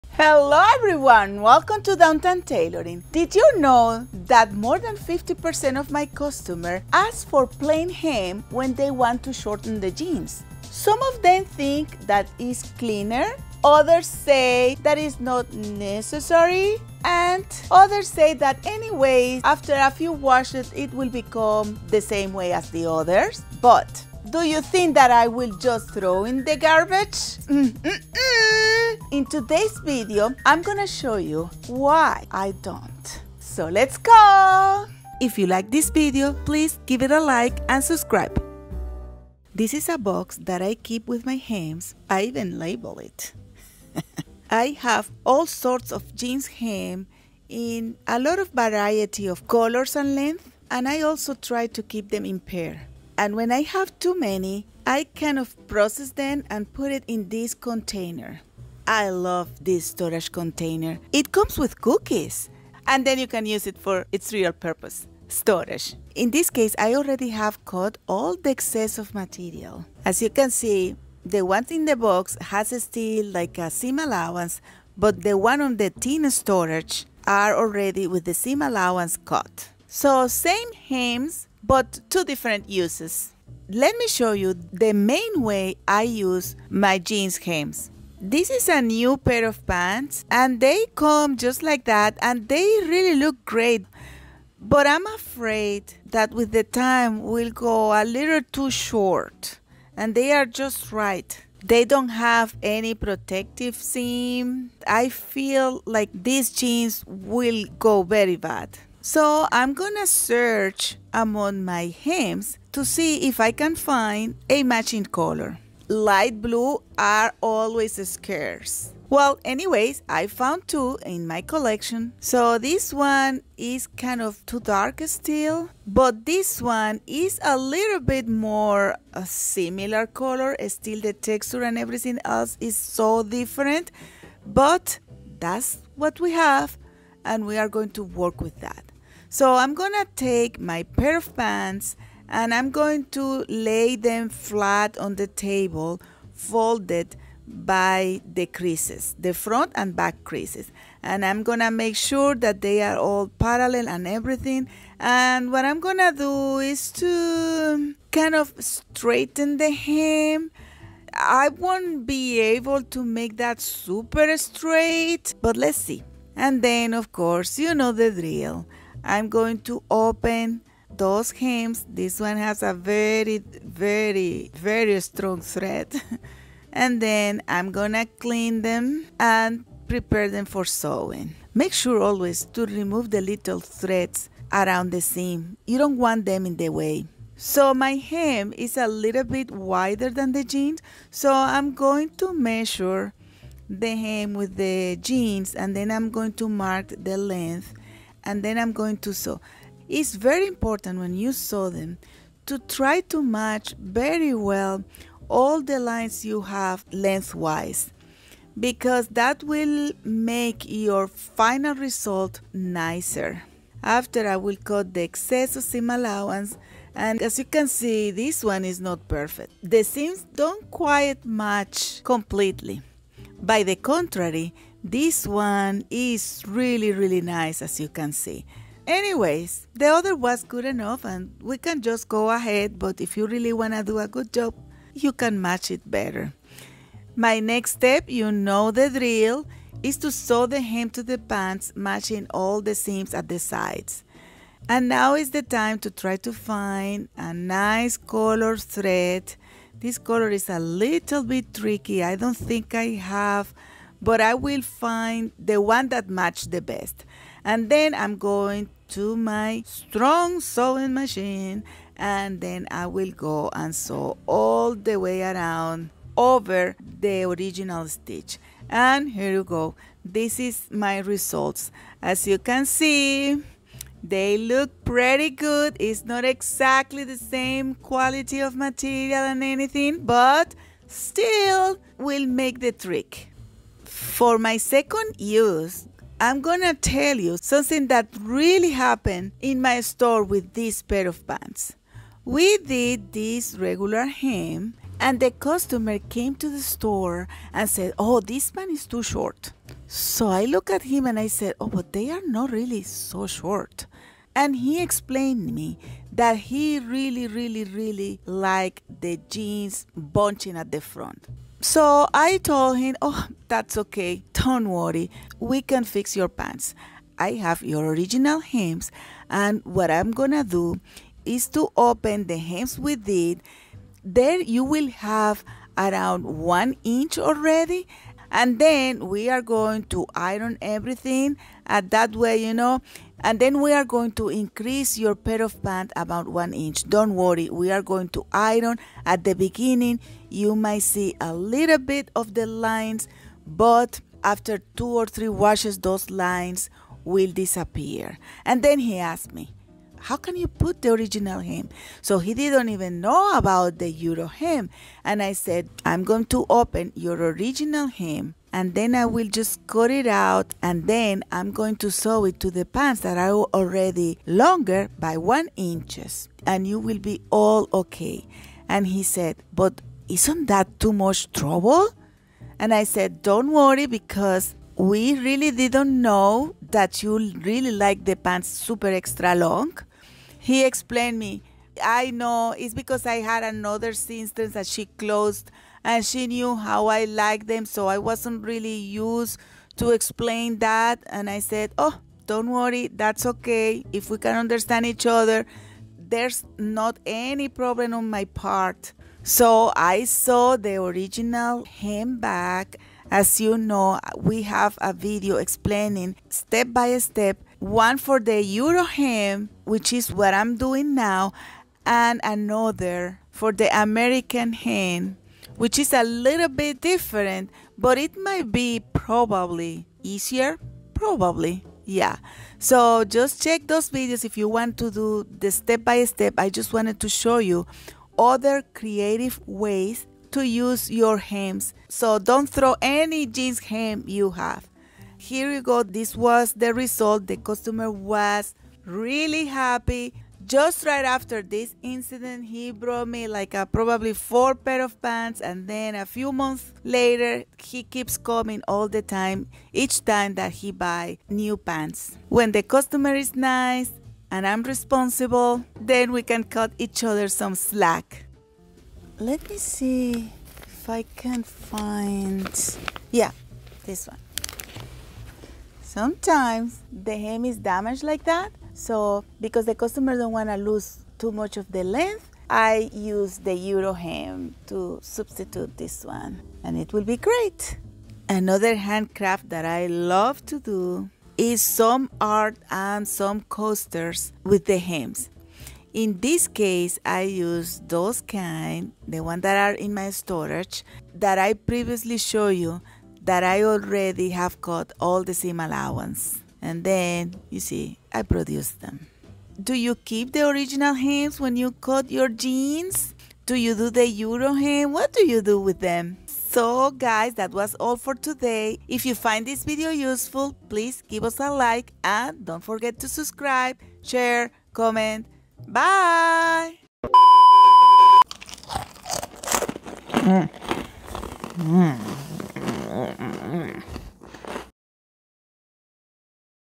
What do you do with leftover jeans hem?? ** Original Hem Technique
Hello friends! In today's video, I wanted to show you some fun ways that I use old leftover jeans hem that I have after doing plain hems! I hope you gain some insight and inspiration!